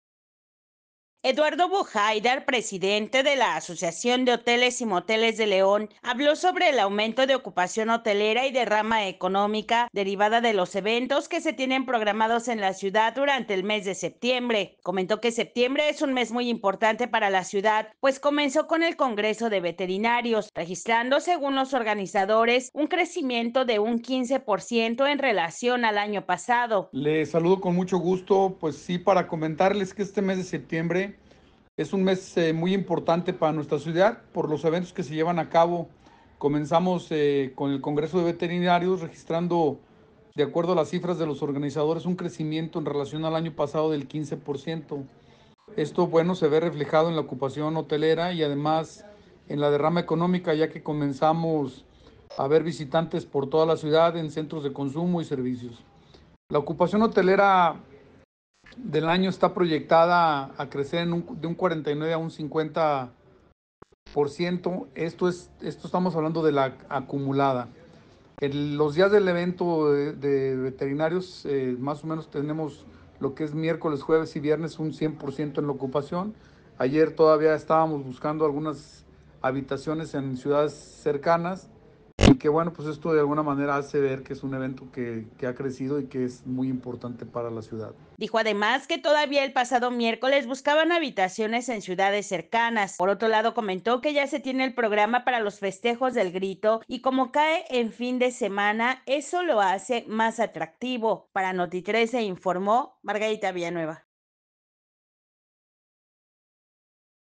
Aumentó la ocupación hotelera y derrama económica en León
VideoNota🎥🔴 | Aumentó la ocupación hotelera y derrama económica en León con los eventos del mes de septiembre.